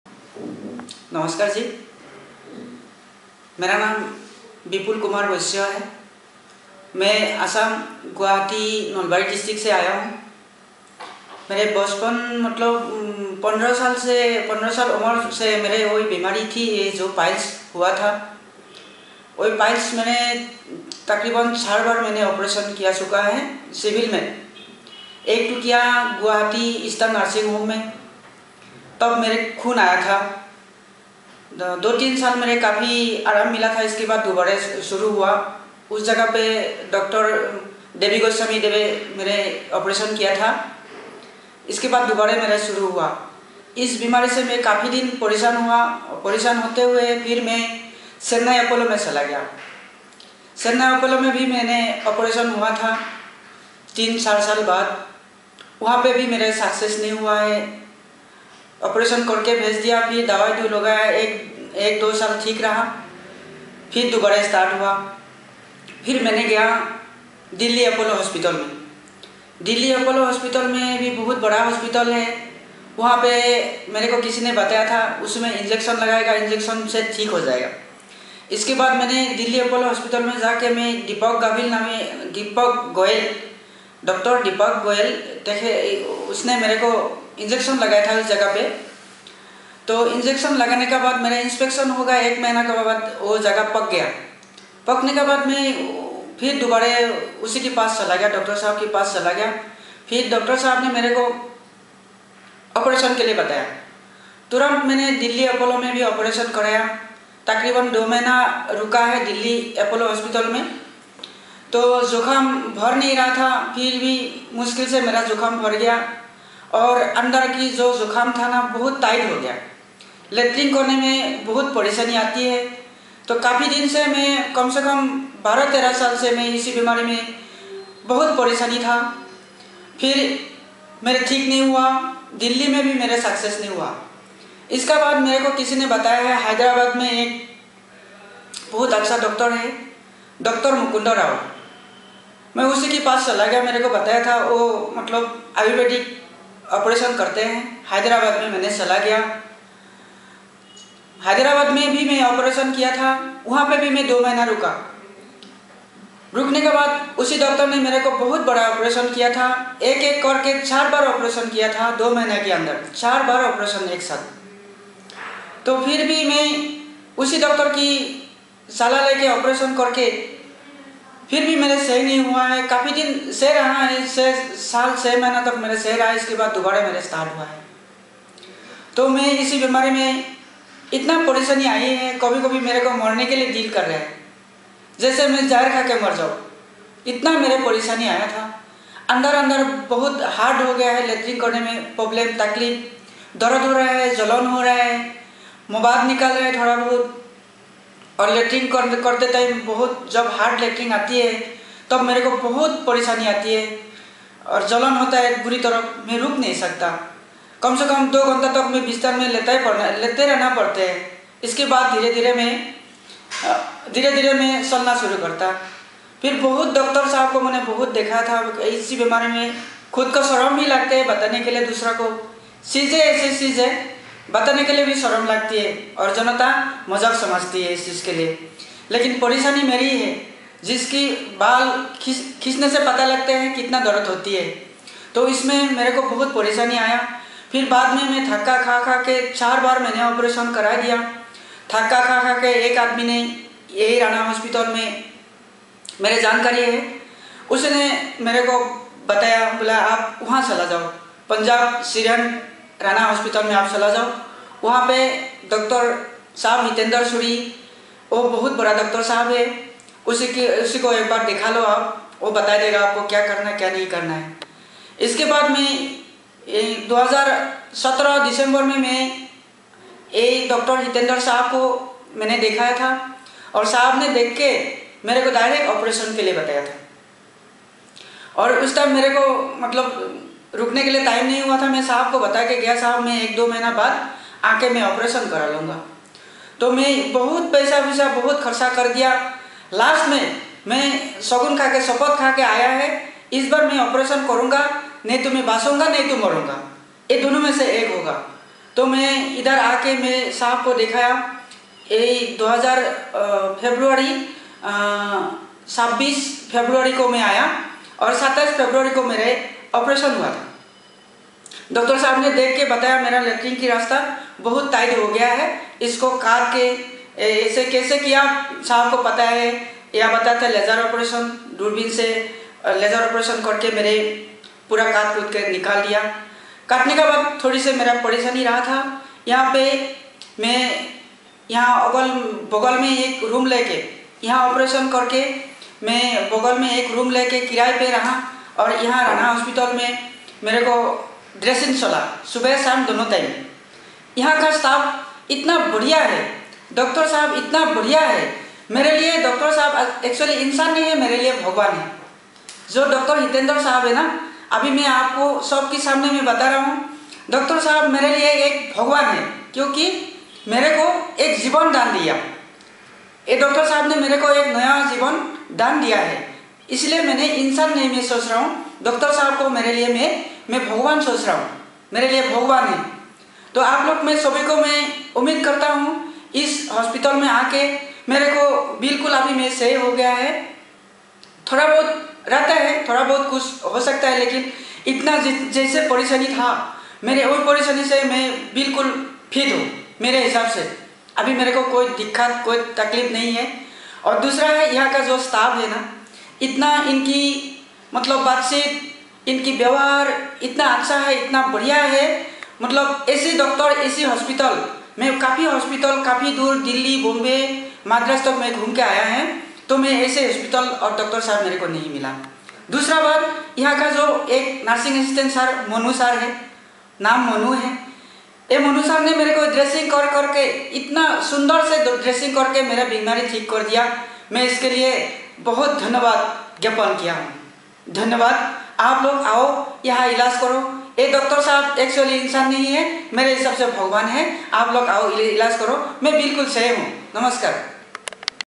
PILES TREATMENT , ANAL STRICTURE TESTIMONIAL BY VIPUL KUMAR FROM ASSAM
In the video, Vipul Kumar introduces himself and shares the problems he was facing due to piles and anal stricture. He then describes his experience at Rana Hospital, praising the hospital's facilities and the professionalism of the staff. He talks about how the staff made him feel comfortable and at ease during the entire treatment process.

Vipul Kumar also speaks highly of the doctors at Rana Hospital, who provided him with accurate diagnosis and treatment plans. He further elaborates on the effectiveness of the treatment, which helped him recover quickly and without any complications.

Towards the end of the video, Vipul Kumar shares his overall experience and highly recommends Rana Hospital to anyone suffering from similar conditions. He encourages people to seek medical help without hesitation and trusts that Rana Hospital will provide them with the best possible care.

This video is a valuable resource for anyone seeking information on piles and anal stricture treatment at Rana Hospital. It provides a firsthand account of the hospital's facilities and the quality of care provided to patients. It is also a testament to the hospital's commitment to delivering excellence in healthcare services.